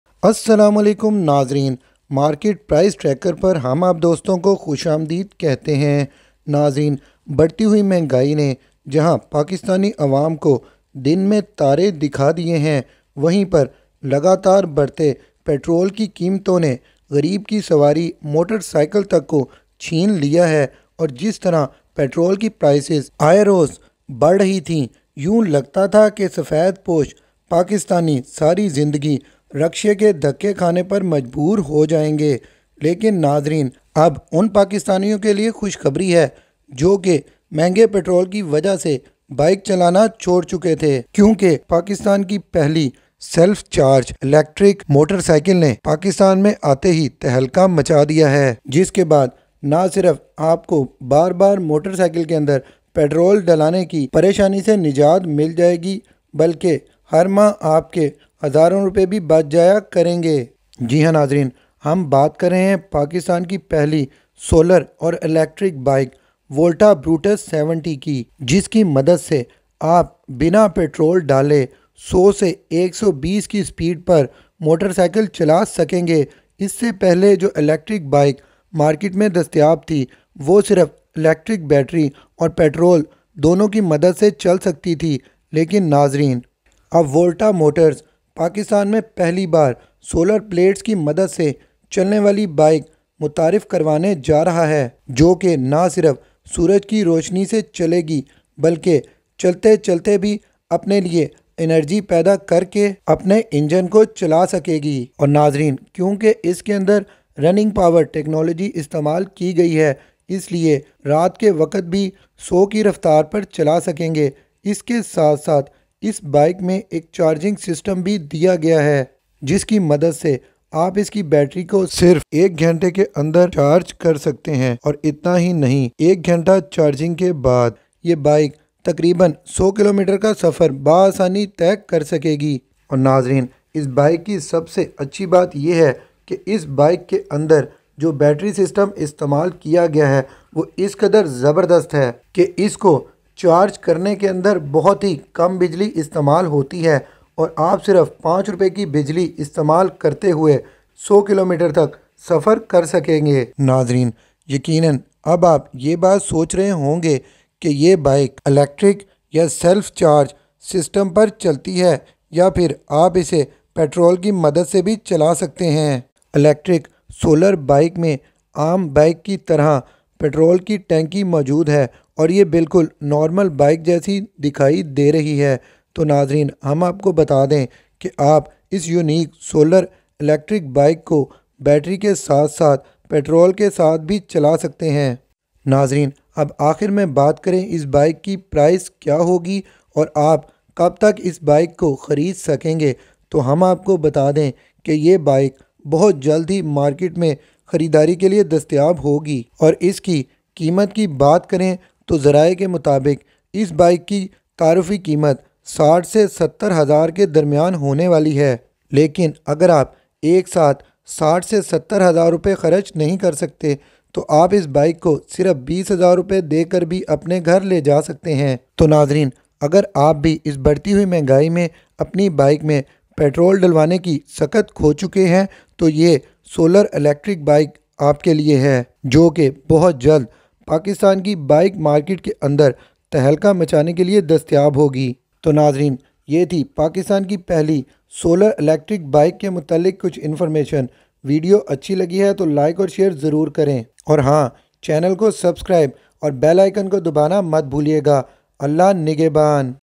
Self Charge Electric Bike Now In Pakistan || Automatic Electric Scooter Technology
Hello friends in this video we going to discuss about pakistan first self charge electric bike which is very effective for 2 person drive .electric e-bike in Pakistan is the hottest topic nowadays. An average Pakistani has to deal with numerous stresses in their everyday lives due to rising prices and limited resources. In particular, rising fuel costs have been a burden on Pakistani citizens as well as those living around the world. Some say that electric bikes could be a potential alternative to cars, especially because a good number of people in Pakistan ride motorcycles or bicycles for their daily commutes.

So many students, delivery workers, job holders, and other professionals have resorted to biking as a cheap and economical option, but wouldn’t it be wiser to invest in an electric bike? If you are looking for the best electric bikes in Pakistan or the best electric motorcycle in Pakistan, this video will help you to choose the best for you.